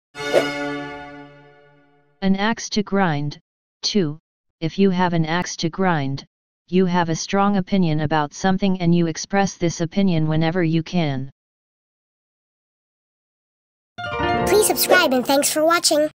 an axe to grind 2 🔊
This series of videos pronounces the American Idioms and defines what they mean. It's vital to understand the most common idioms in order to avoid absolute confusion.